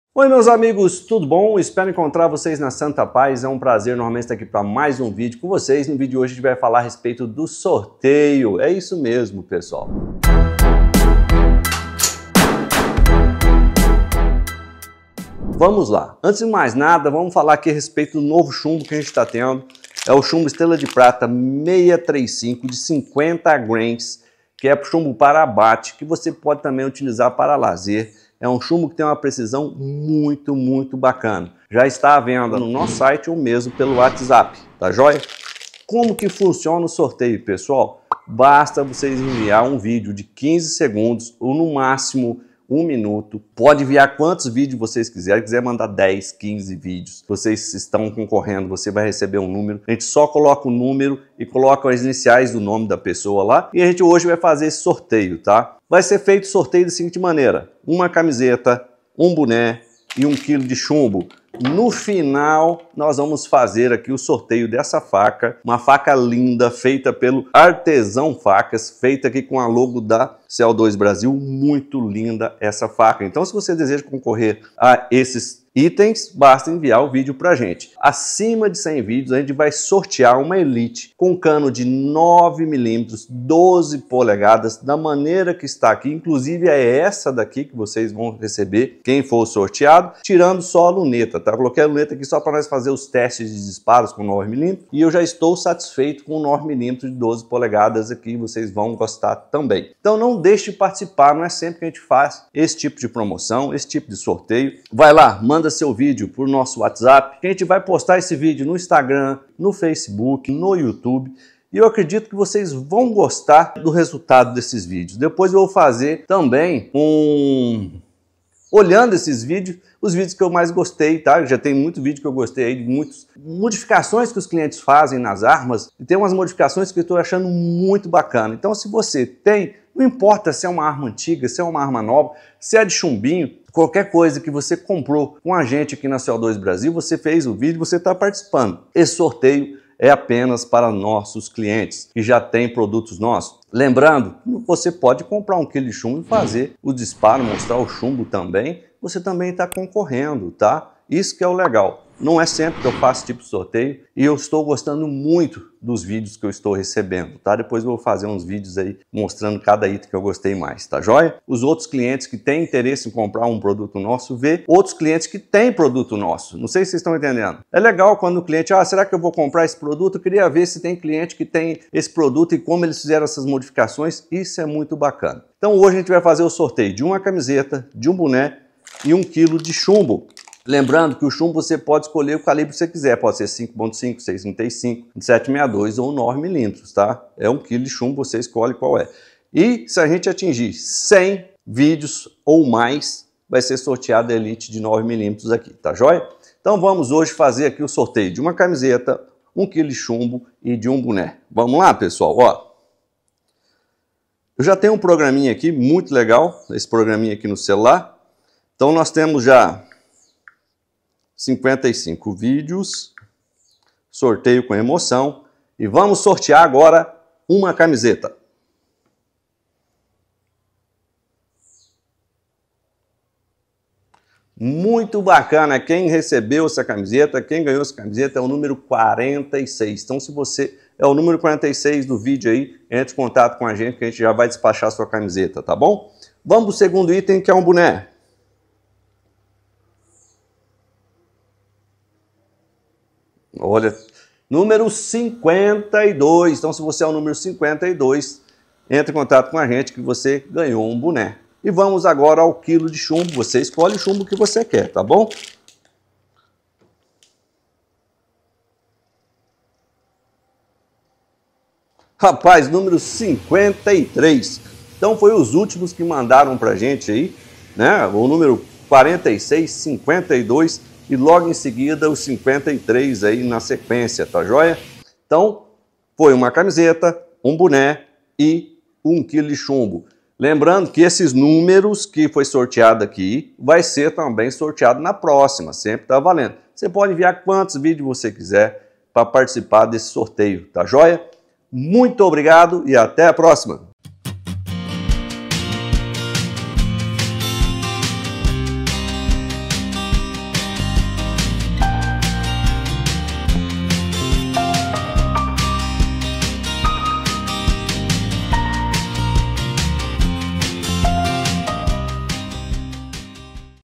Elite 9mm Sorteio para Clientes - Parte 1
O vídeo trata de um sorteio para clientes da CO2 Brasil, onde o prêmio é uma camiseta, um boné e um quilo de chumbo Estrela de Prata 6.35 de 50 grains, além da possibilidade de concorrer a uma arma Elite de 9mm com cano de 12 polegadas. Para participar do sorteio, os clientes devem enviar um vídeo de 15 segundos ou um minuto, podendo enviar quantos vídeos quiserem. Os números sorteados foram 46 para a camiseta, 52 para o boné e 53 para o quilo de chumbo, e os ganhadores devem entrar em contato para receber seus prêmios. O chumbo Estrela de Prata 6.35 já está à venda no site e pelo WhatsApp da empresa. O sorteio é exclusivo para clientes da CO2 Brasil e o organizador agradece a participação de todos.